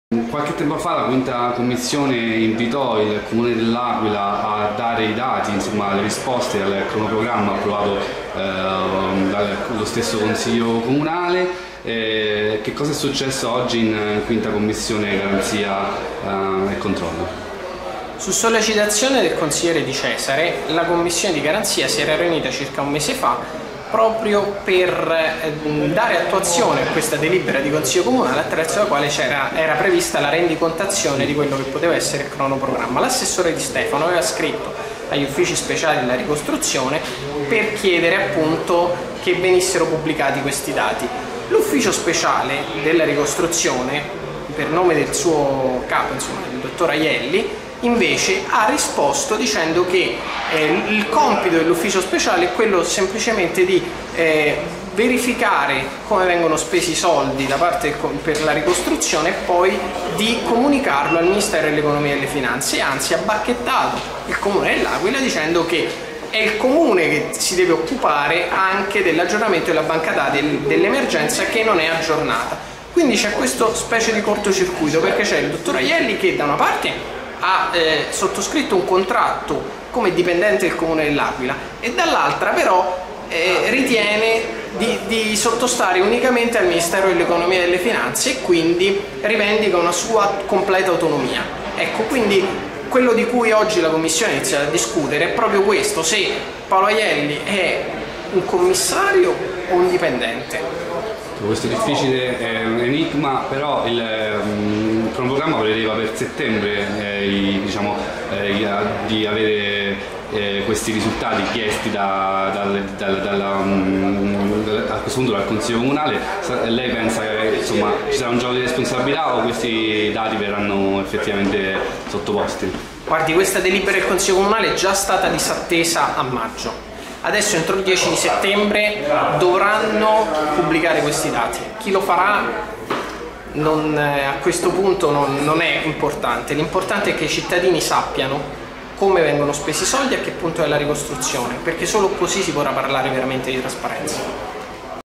Daniele, Presidente V Commissione su Aielli e rendiconto Cronoprogramma
5/9/2013 L'Aquila